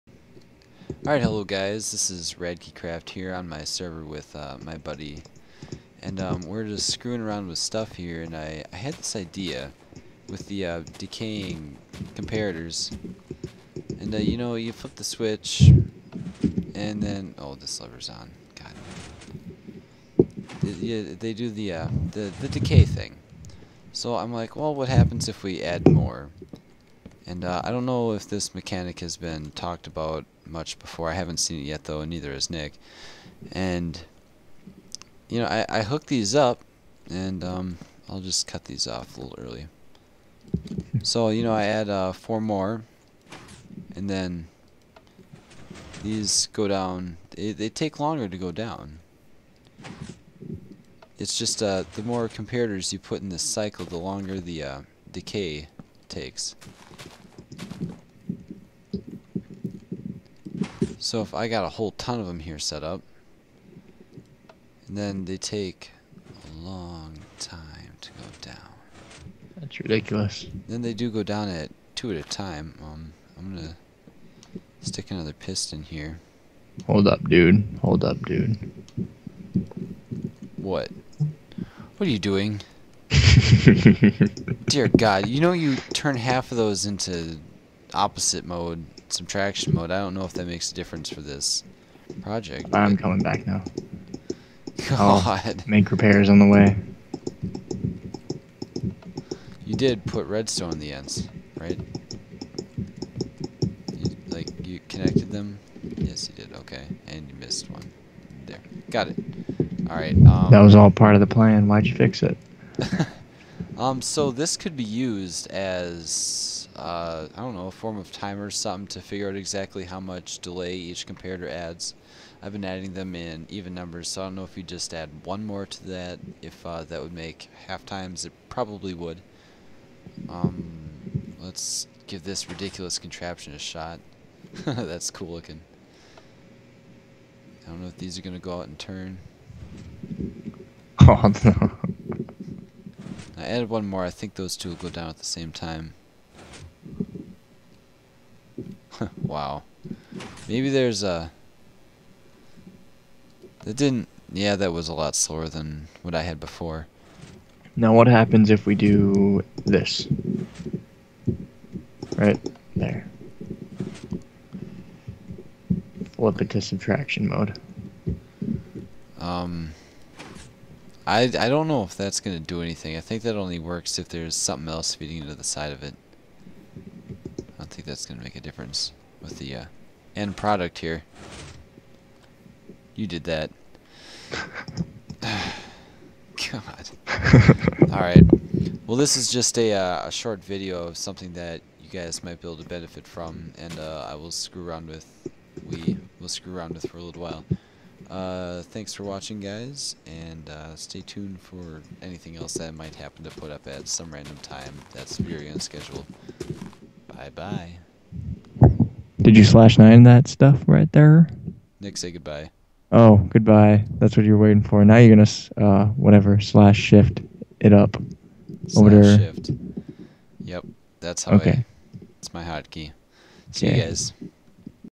Redstone Comparator Decay
While derping around on my redstone-oriented creative server, I discovered something regarding the rate at which signal decay occurs while using redstone comparators.